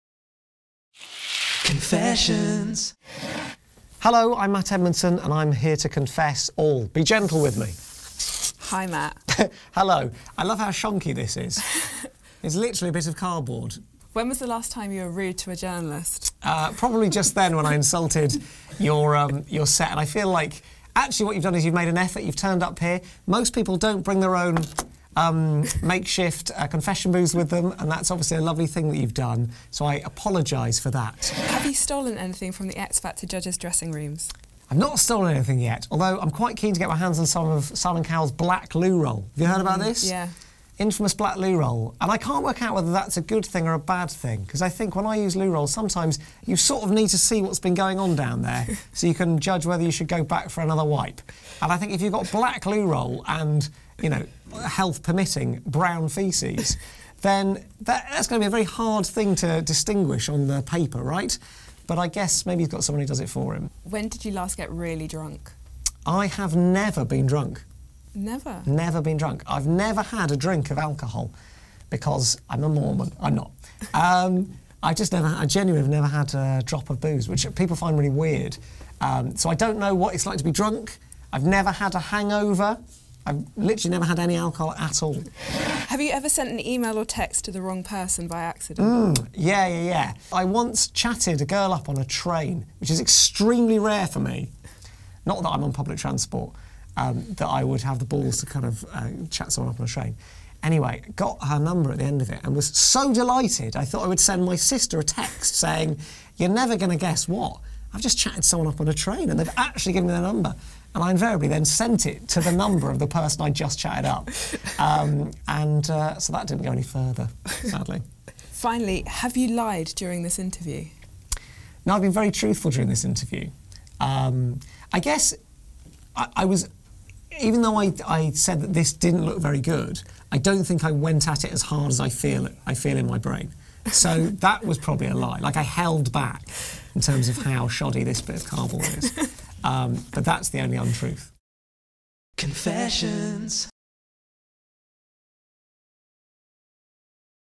Matt Edmondson on Simon Cowell's loo roll and chat-up fails
Matt Edmondson enters the Digital Spy Confessions booth, and after he was done slagging off our set, he told us that he's eyeing up a roll of Simon Cowell's infamous black toilet paper.

Matt also tells us about the time he successfully chatted up a girl only to blow it by accidentally bragging about it to her!